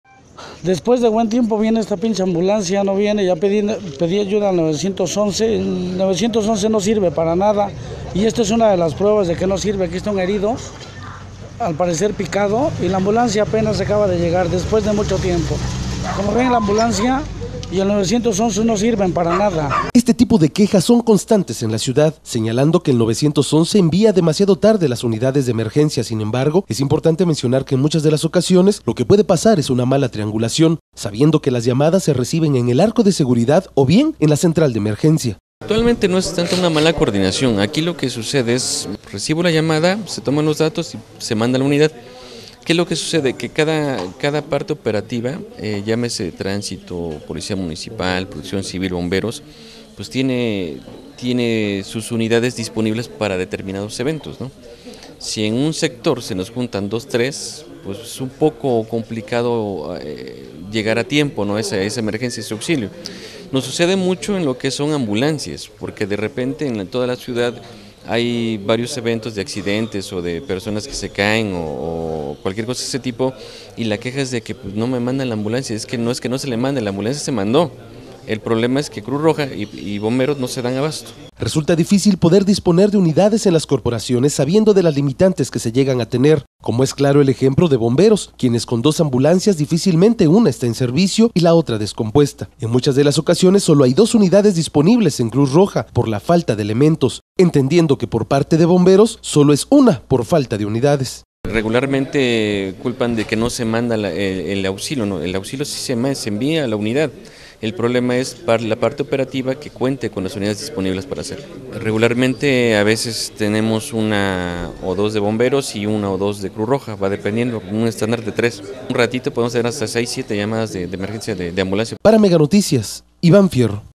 TEH Insuficientes ambulancias para servicios en el municipio
(Spanish News) Noticias
Gracias Por El Soporte No Seles Olvide Subscribir Mi Canal Gustar Y Compartir Gracias Tengan Buen Dia.